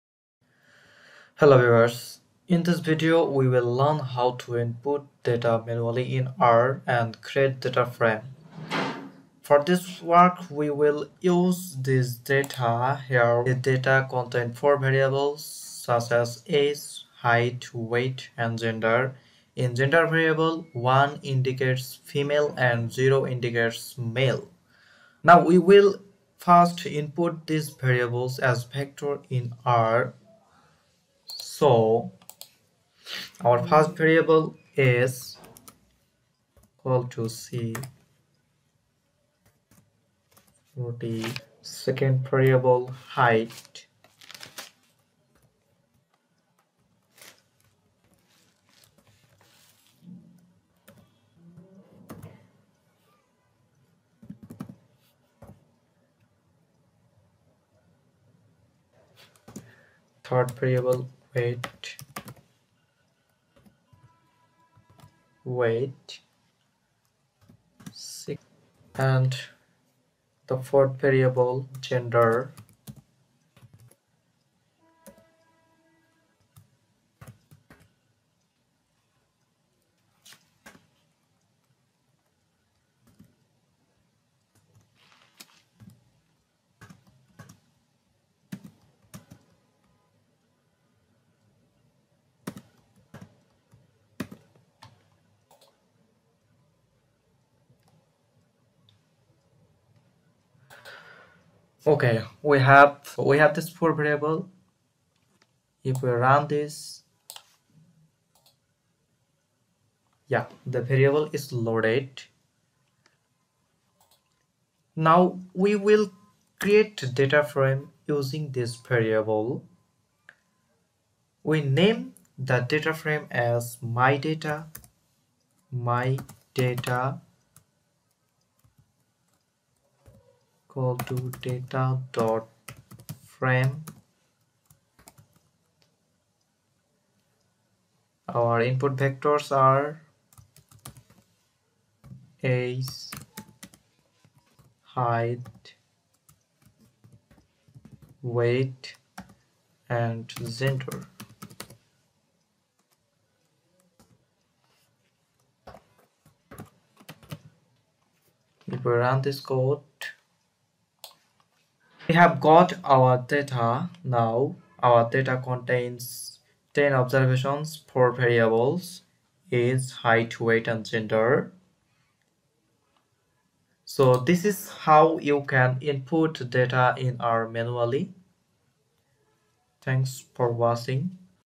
How To Input Data Manually In R and Create Data Frame
A data frame is a collection of vectors of equal length. A matrix only allows one type of data, but a data frame accepts multiple data types (numeric, character, factor, etc.).